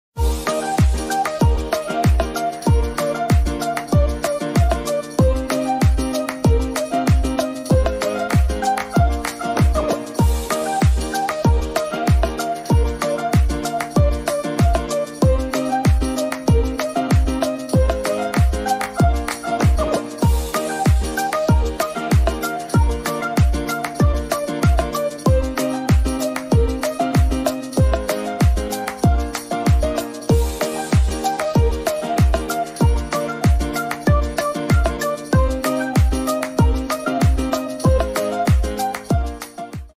Dubai Gulf Food Manufacturing 2023 High lights Review
Celebrate the successful conclusion of this Dubai show and join us for an exclusive look back at the highlights from Gulf Food Manufacturing 2023 in Dubai and take a look at the latest food packaging innovations!